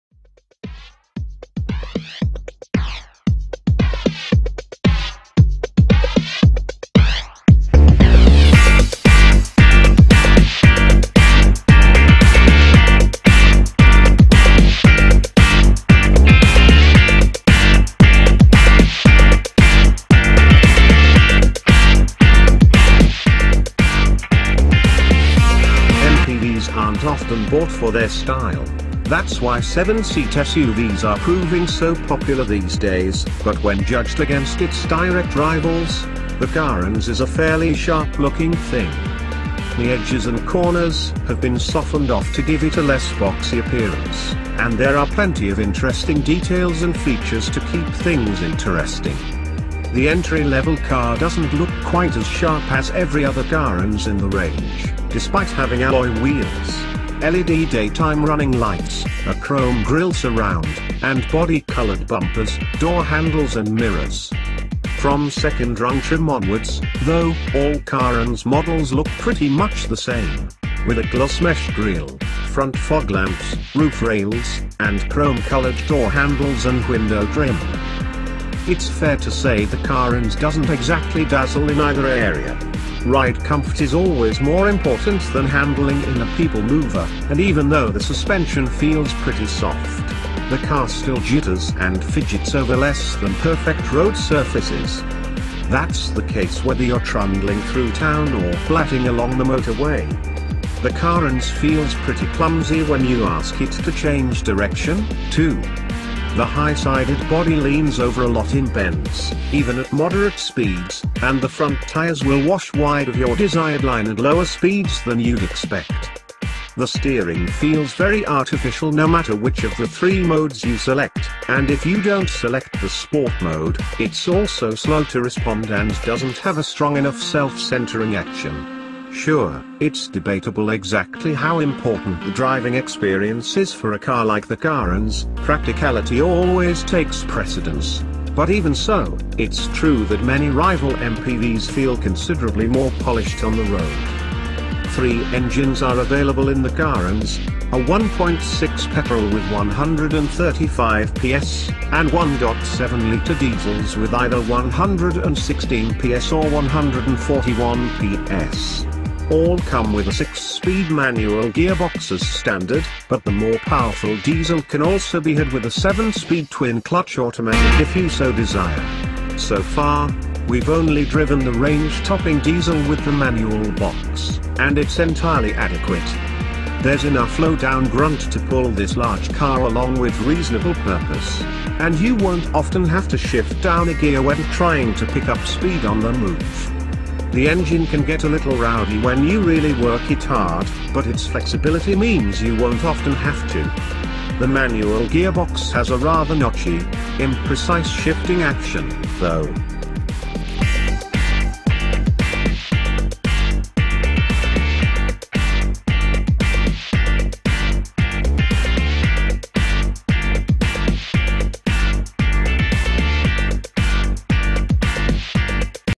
[BEST MPV] Kia Carens !!!
MPVs aren’t often bought for their style – that’s why seven-seat SUVs are proving so popular these days – but when judged against its direct rivals, the Carens is a fairly sharp-looking thing. The edges and corners have been softened off to give it a less boxy appearance, and there are plenty of interesting details and features to keep things interesting. The entry-level car doesn’t look quite as sharp as every other Carens in the range, despite having alloy wheels, LED daytime running lights, a chrome grille surround, and body-coloured bumpers, door handles and mirrors. From second-rung trim onwards, though, all Carens models look pretty much the same, with a gloss mesh grille, front foglamps, roof rails, and chrome-coloured door handles and window trim.